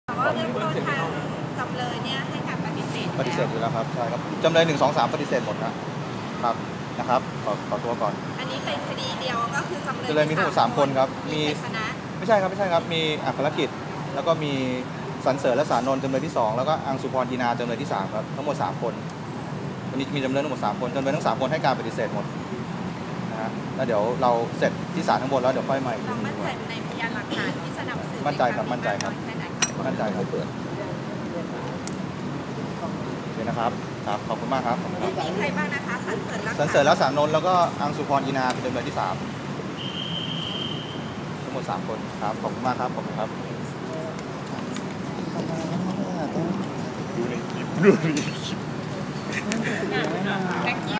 ทนายเบนซ์
เมื่อเวลา 08.00 น. นายสิทธิโชค ตรีเนตร ทนายส่วนตัวของเบนซ์ เรซซิ่ง เดินทางมาถึงศาลอาญา เพื่อนัดตรวจหลักฐานคดีฟอกเงินและสบคบฟอกเงิน ของจำเลยทั้ง3ได้แก่ นายอัครกิตติ์ วรโรจน์เจริญเดชหรือเบนซ์ เรซซิ่ง, นายสรรเสริญ หรือแน็ต รสานนท์, น.ส.อังสุพร หรืออุ้ม อินา ซึ่งจำเลยทั้ง3ให้การปฏิเสธ และมั่นใจในพยานหลักฐานในครั้งนี้ ทั้งนี้ทนายจะให้การสัมภาษณ์กับสื่อมวลชนอีกครั้งหลังจากนี้เวลาประมาณ 9.30 น.